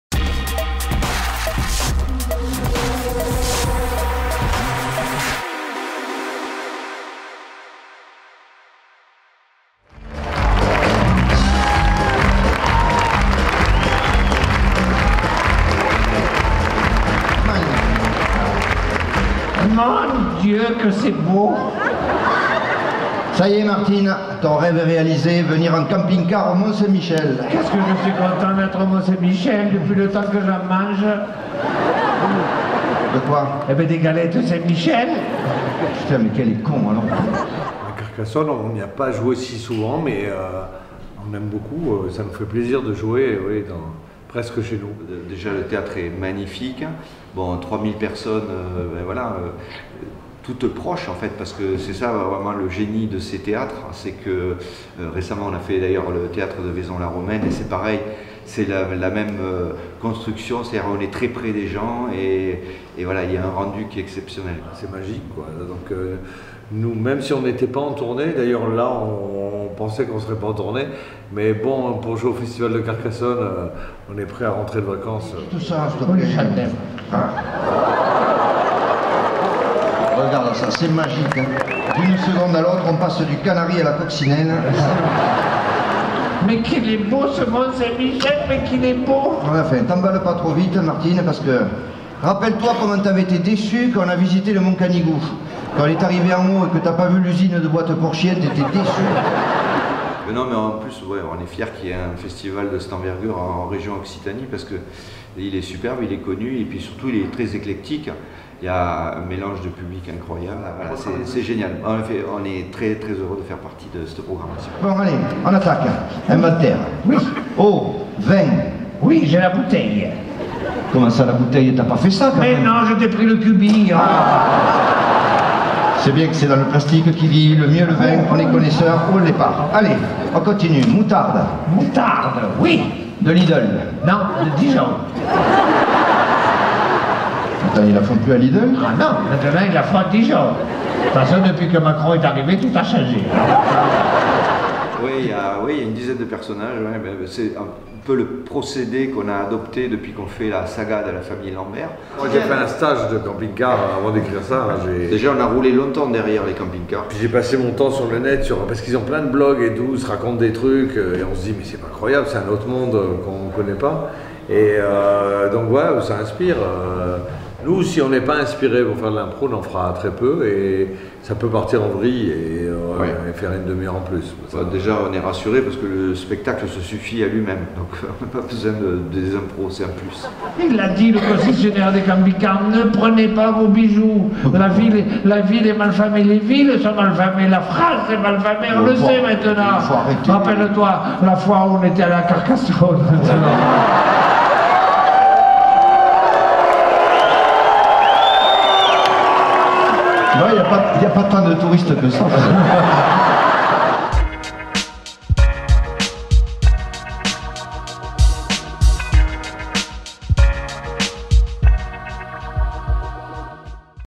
L'instant Festival : Les Chevaliers du Fiel
C'est devant un Théâtre Jean-Deschamps comble que Les Chevaliers du Fiel ont fait une halte de leur Camping-Car au Mont Saint-Michel... de Carcassonne, pour le plus grand plaisir du public !
▶️ Rencontre exclusive avec Eric Carrière et Francis Ginibre, deux Chevaliers made in Occitanie, ravis de jouer au Festival de Carcassonne

retrouvez-nous sur Facebook, Twitter, Instagram, Snapchat